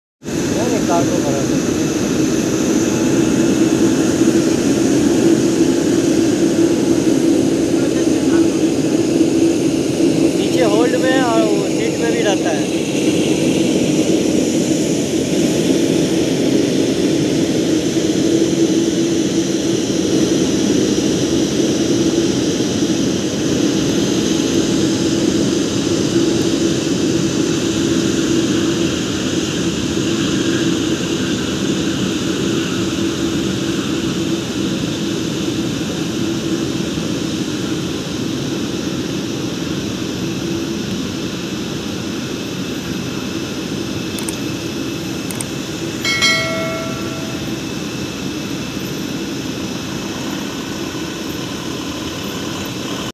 Qater Airways Boeing-787-8 Dreamliner At Kolkata International Airport ✈️🇮🇳
Qater Airways Boeing-787-8 At Kolkata International Airport.

World's best airplane dreamliner Beoing 787_8 at Kolkata international airport..